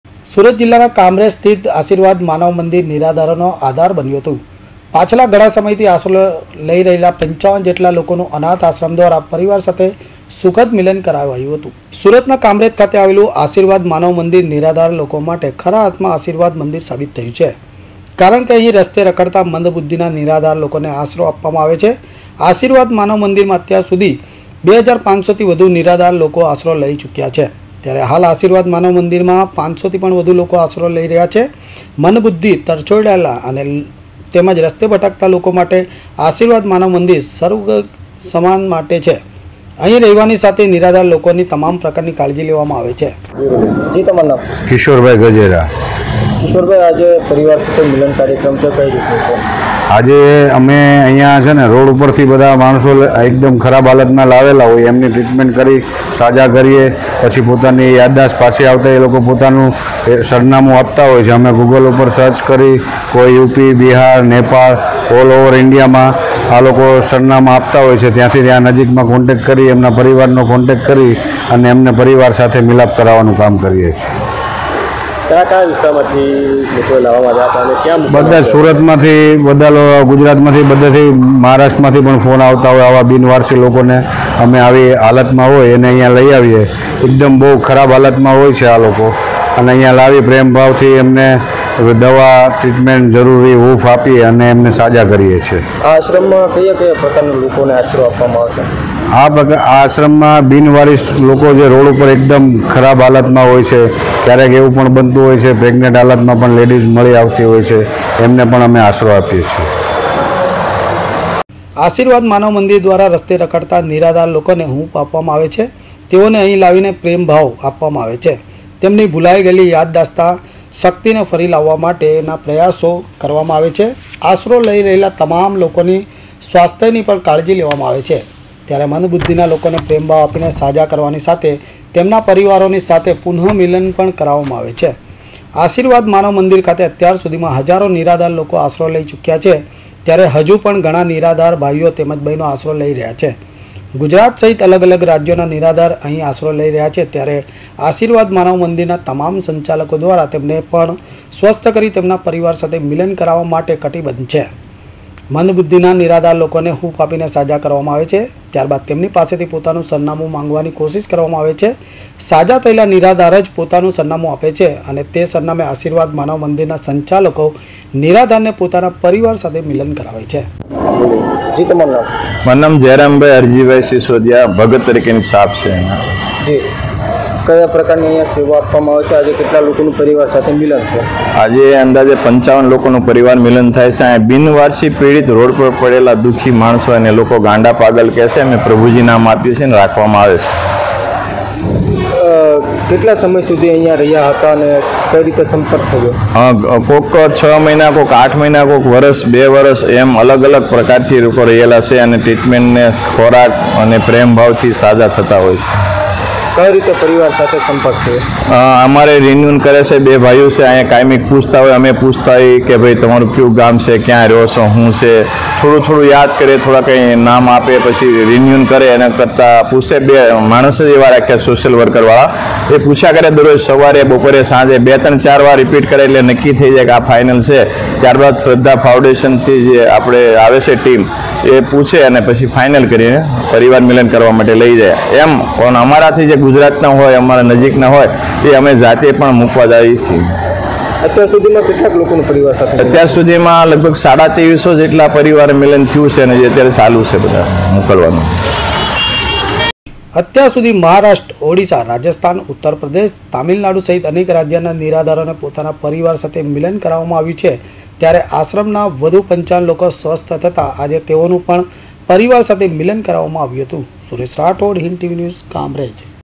નીરાઘારો નો આધાર બન્યું કામરેજ નું આશીર્વાદ માનવ મંદિર
55 જેટલા લોકોનું પરિવાર સાથે મિલન કરાવવામાં આવ્યું
ઘણા સમયથી આશરો લઈ રહેલા લોકોનું પરિવાર સાથે સુખદ મિલન કરાયું
હાલ આશીર્વાદ માનવ મંદિરમાં 500થી પણ વધુ લોકોને આશરો અપાયો